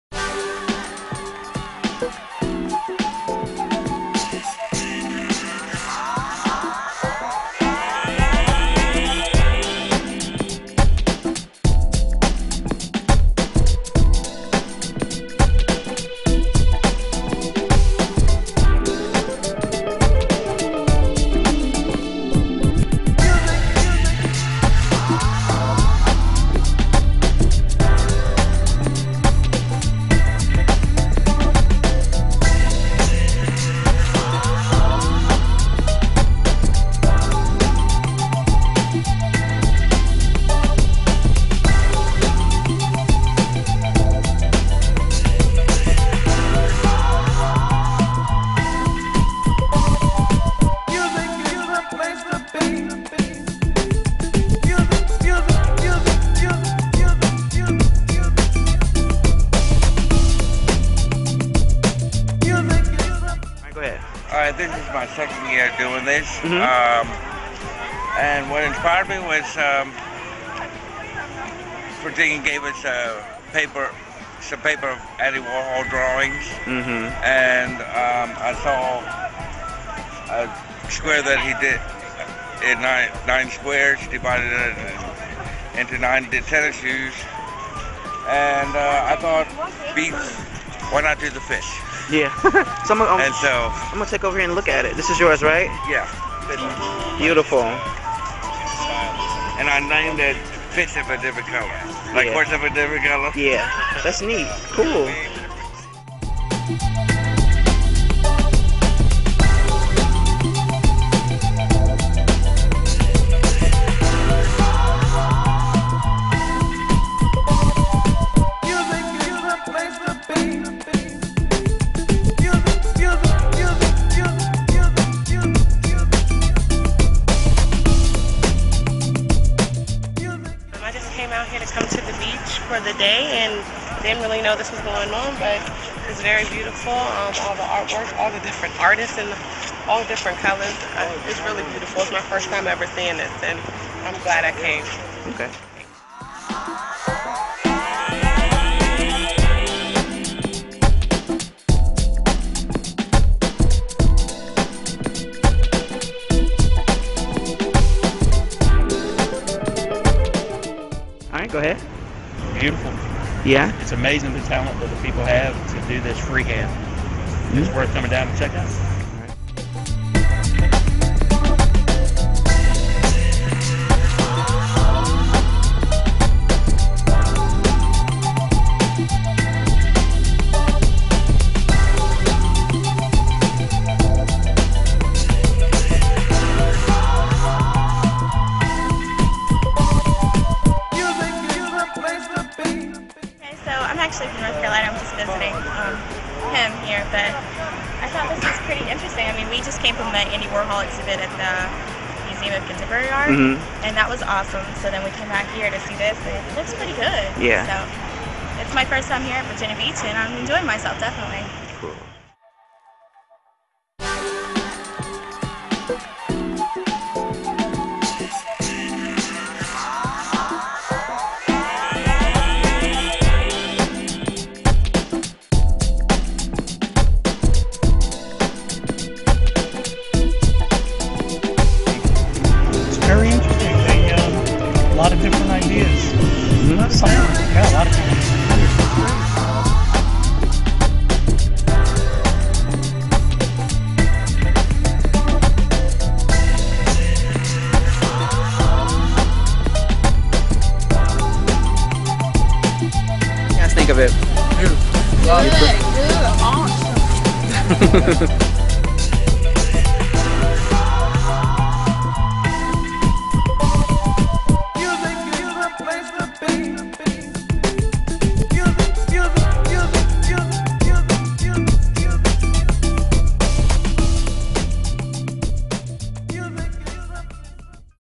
Chalk the Walk
Face of Fox, Tracie Paige, walked the Virginia Beach Board Walk to peep the artwork at the 2012 Chalk the Walk Artsplosion!